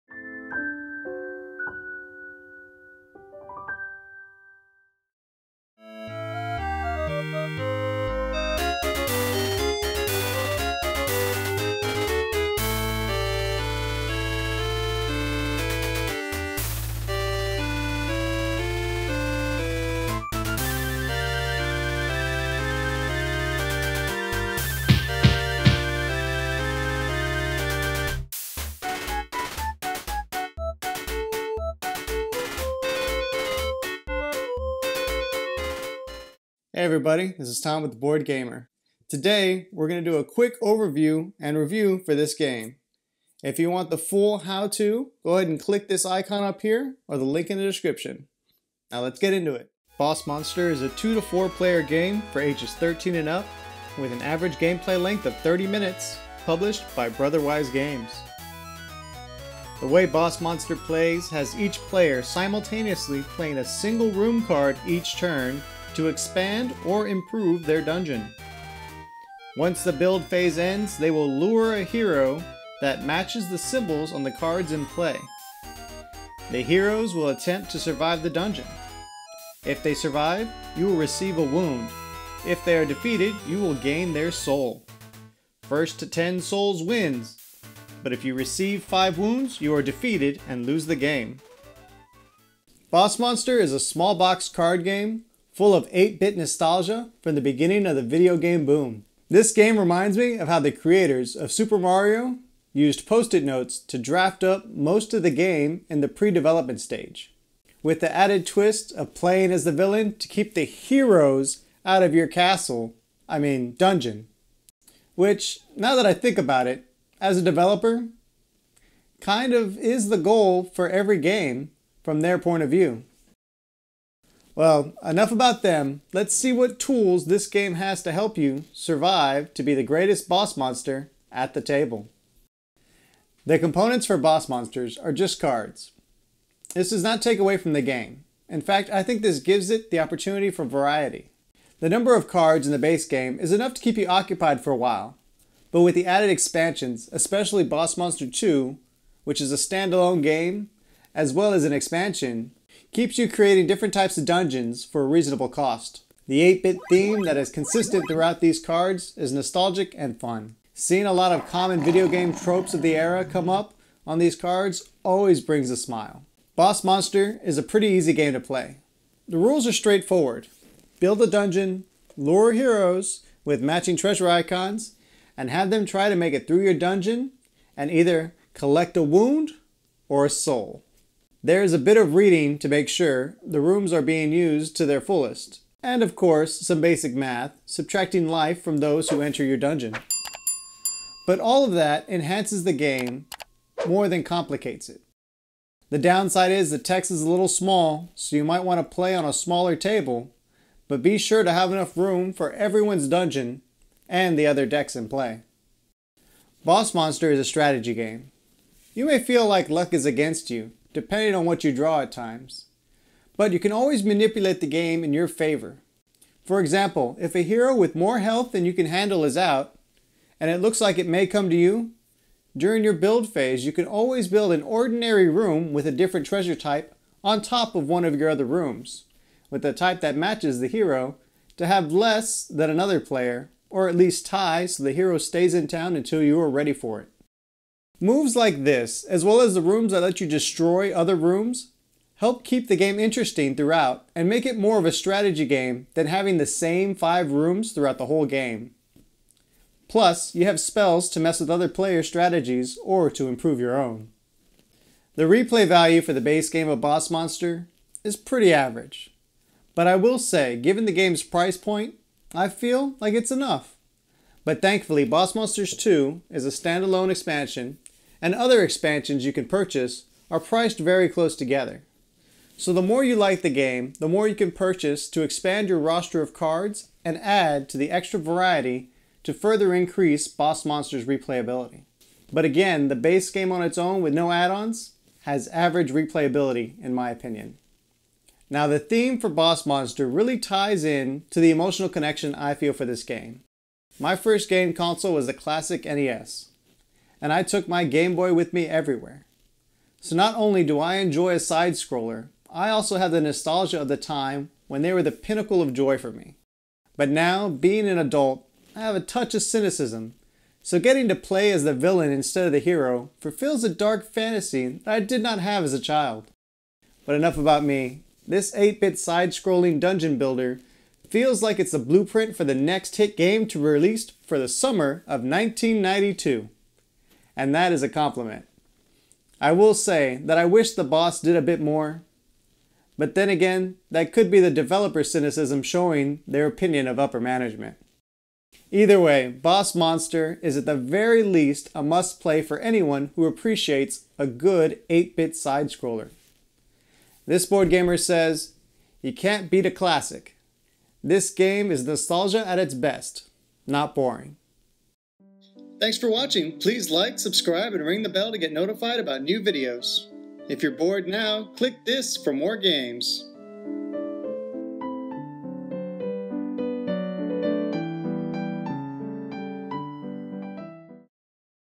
Boss Monster Review
If you have not tried Boss Monster yet watch this video and find out why you should. This 8 Bit nostalgia filled card game takes you back to a time where you had to really strategize to neat a dungeon. However the twist is you are the one building the dungeon. Place those traps and monsters to stop any hero from getting through. Become the greatest Boss Monster of the land.